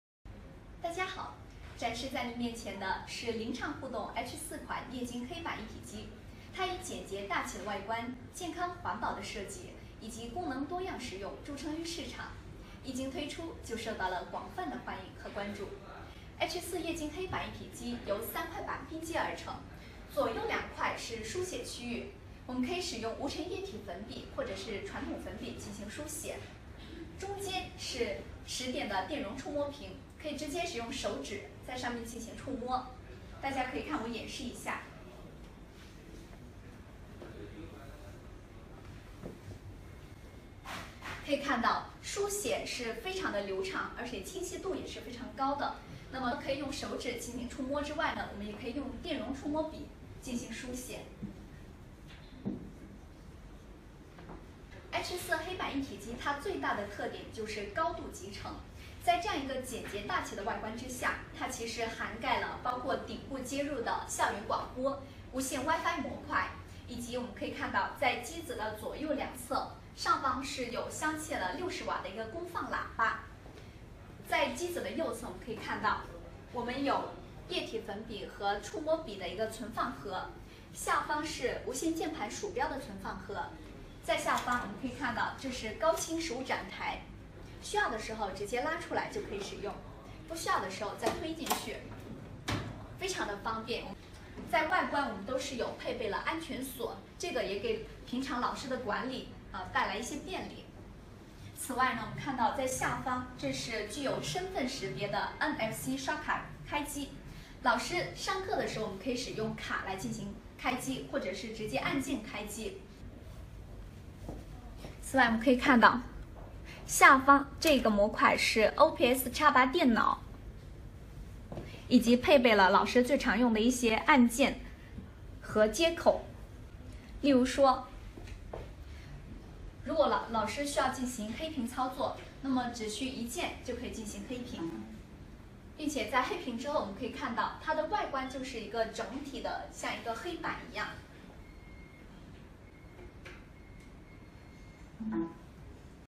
Introduction of Smart E-blackboard
1.Integrated design( integrated with LED touch screen, blackboard, visualizer, centralized control system, wireless mouse, efficacy, speaker etc.)
2.Humanization design ( the perfect combination of tradition and technology, one key switch, preposition speaker with wireless MIC, reducing burden of teacher's voice)